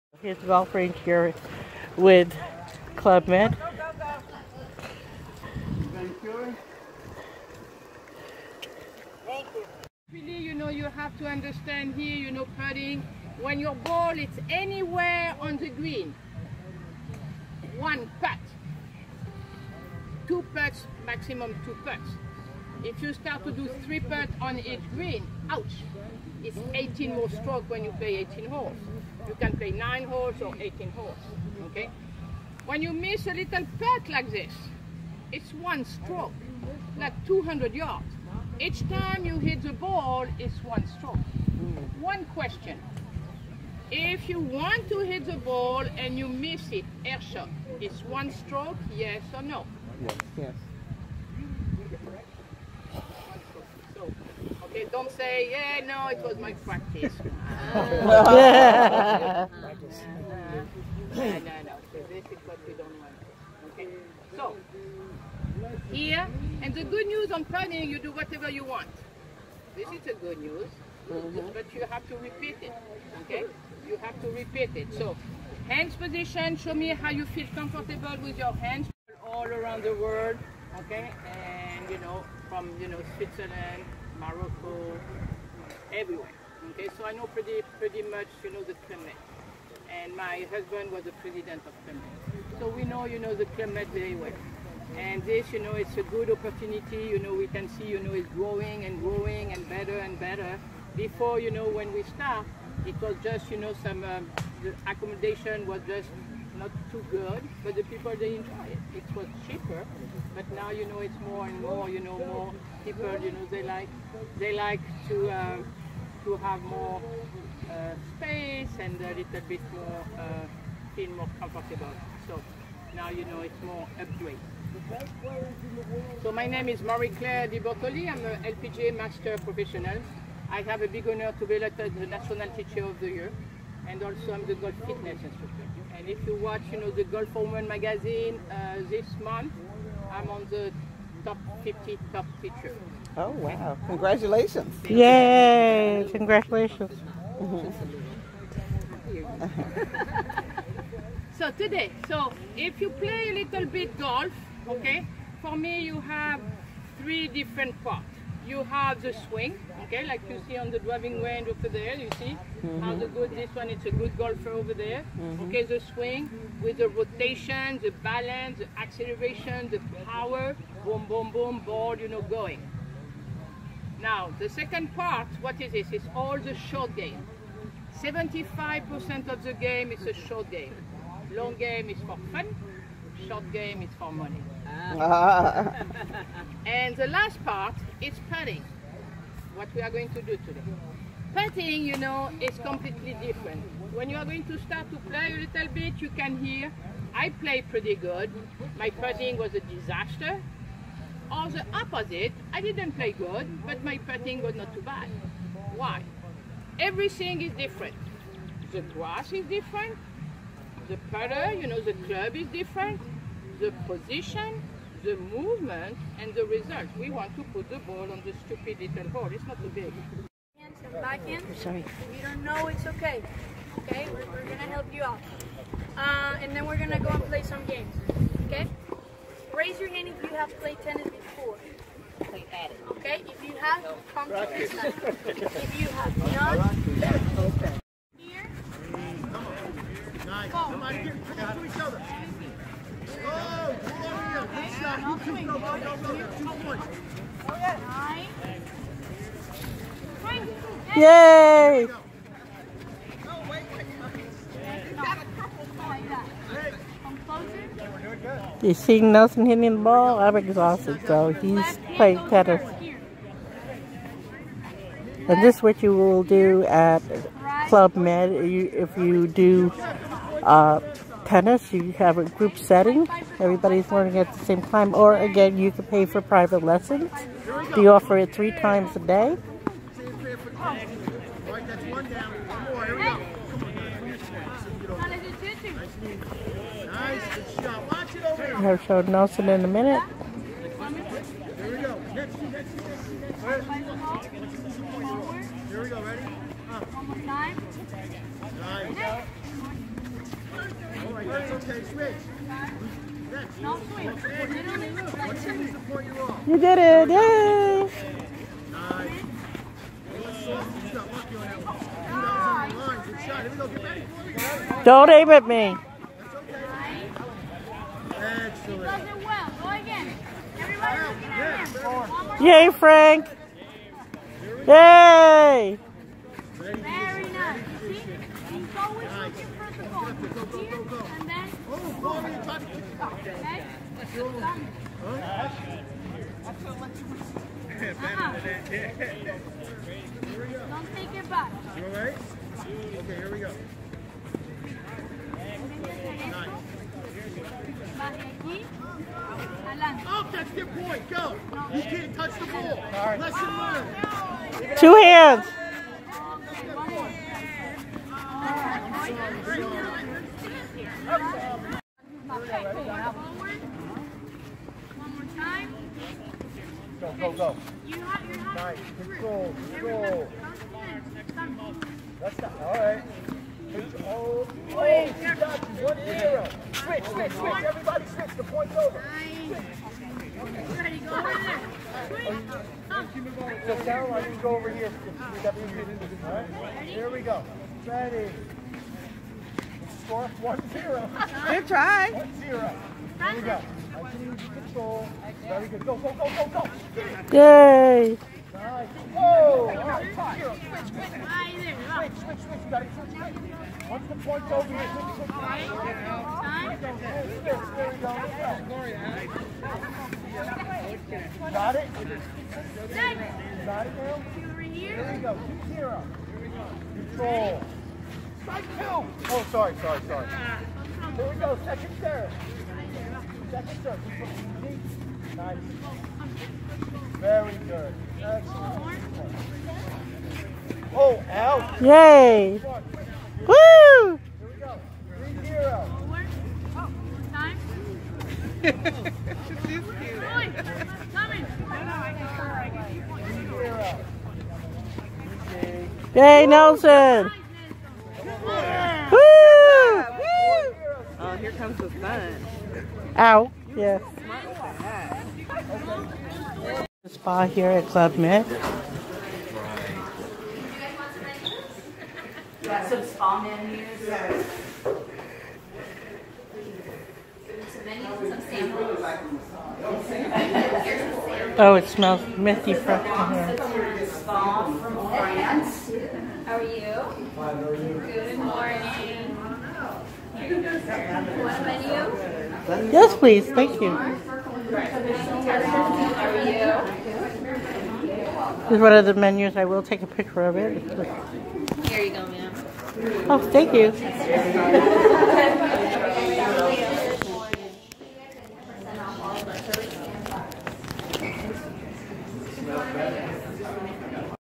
Club Med Sandpiper Bay, Part 2 February 2018
Club Med Sanpiper Bay is full of all inclusive activities from Group Tennis, Golf, Volleyball, even pickelball. All free, if you want to play on your own, you have to play for services.  Nelson and I, took golf, they have Marie Claire De Bortoli who in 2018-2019 named 50 top LPGA teachers in the USA. So you are learning from the best. We also played Tennis, yes even us legally blind people can play golf and tennis.

For more information on any of the 40+ Club Meds around the world. Please contact your favorite travel adviser Cheryl Echevarria.